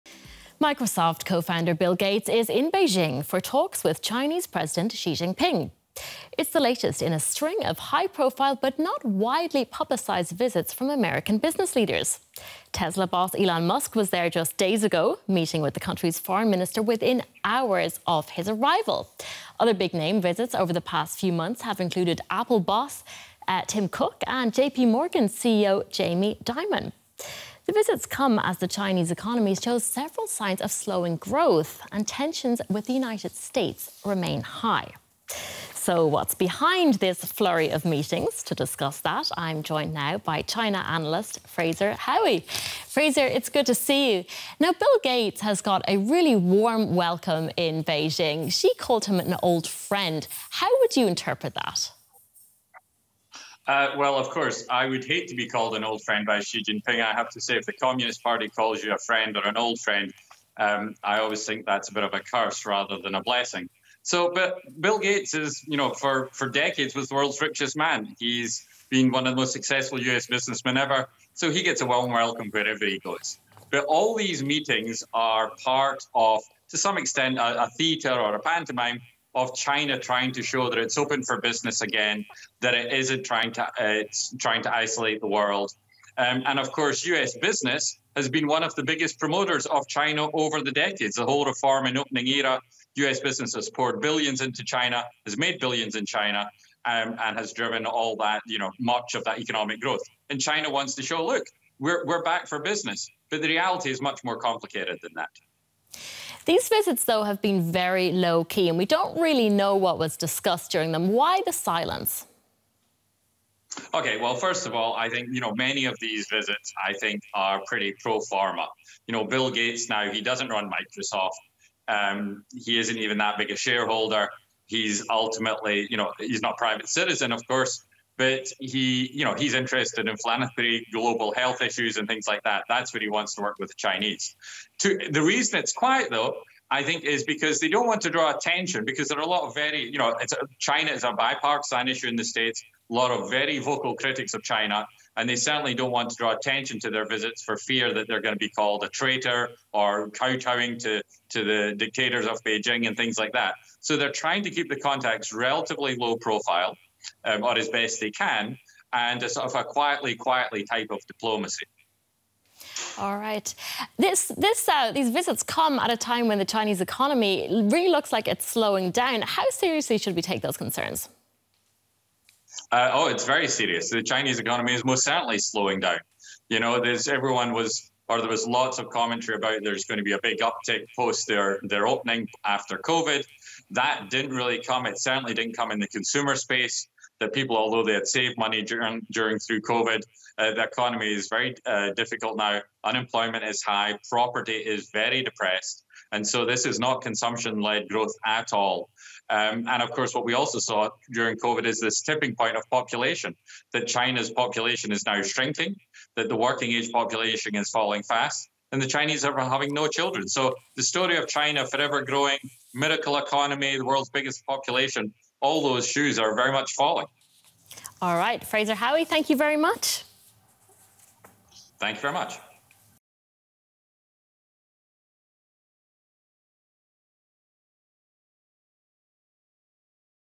China pushes message that it’s open for business as the economy shows signs of flagging | DW News
Microsoft Co-Founder Bill Gates heads to Beijing for talks with Chinese President Xi Jinping. It's the latest in a string of high-profile visits, including Tesla boss Elon Musk and Apple boss Tim Cook. The visits come as the Chinese economy shows signs of slowing growth and tensions with the US remain high.